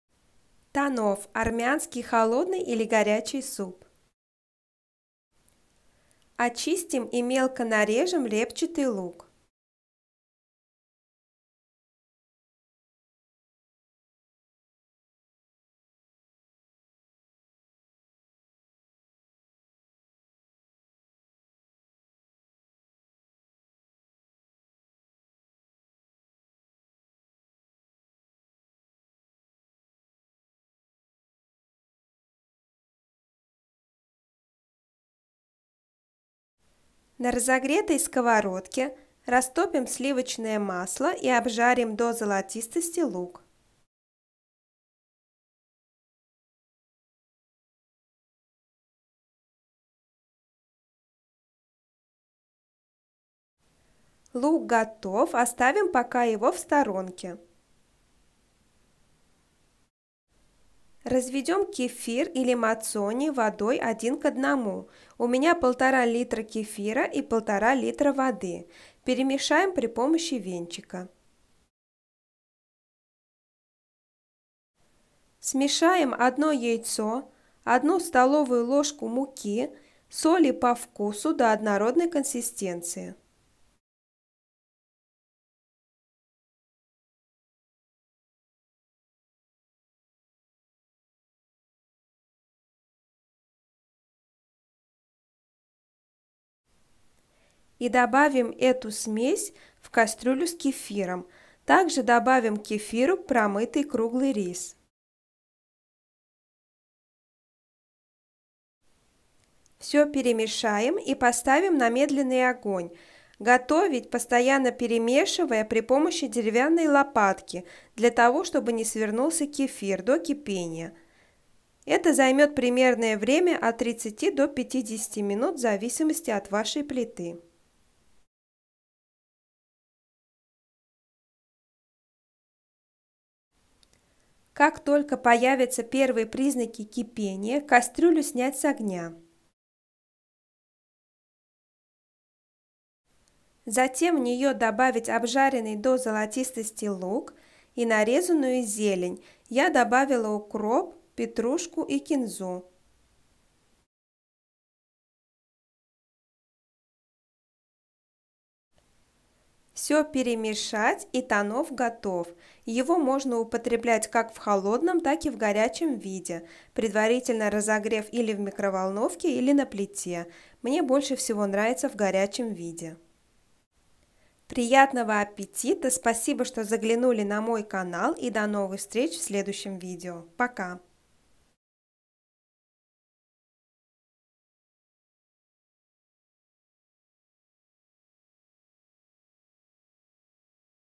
ТАНОВ / ХОЛОДНЫЙ ИЛИ ГОРЯЧИЙ СУП | СУПЫ | ПРОСТЫЕ РЕЦЕПТЫ
Ингредиенты:

1,5 л кефира или мацони
1,5 л воды
1 репчатый лук
30-50 гр сливочного масла
1-2 горсти круглого риса
1 яйцо
1 ст л муки
Зелень и соль по вкусу

Приятного аппетита!